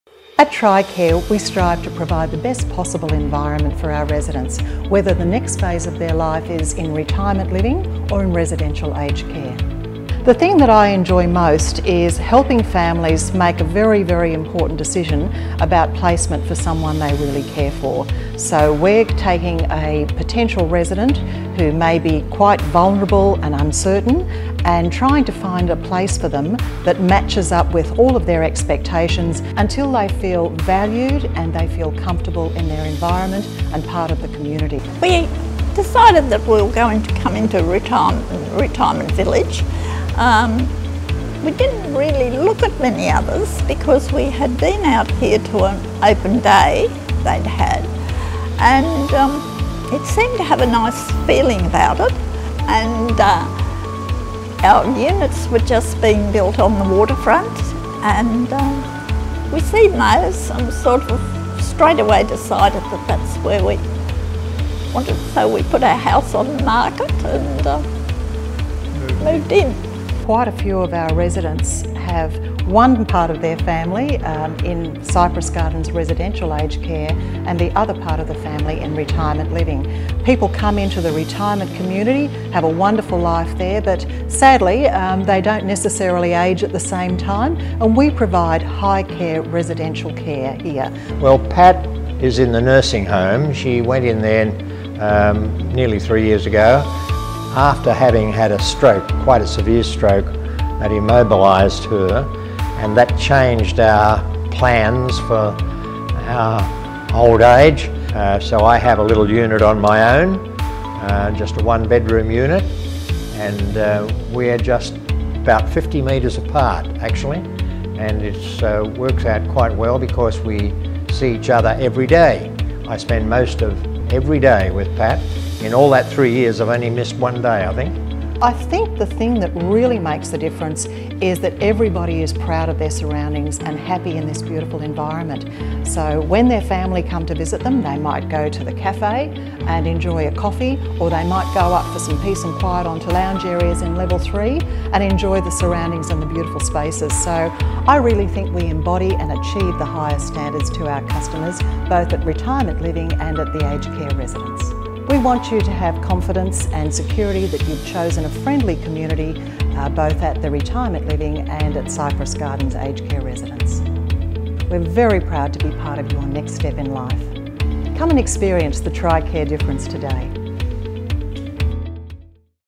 TriCare Cypress Gardens Retirement and Aged Care Co-location
Cypress Gardens is luxury aged care, co-located with Cypress Gardens Retirement Village. It's handy for couples with individual accommodation and care needs and offering a wide range of comforting care and hotel-style accommodation.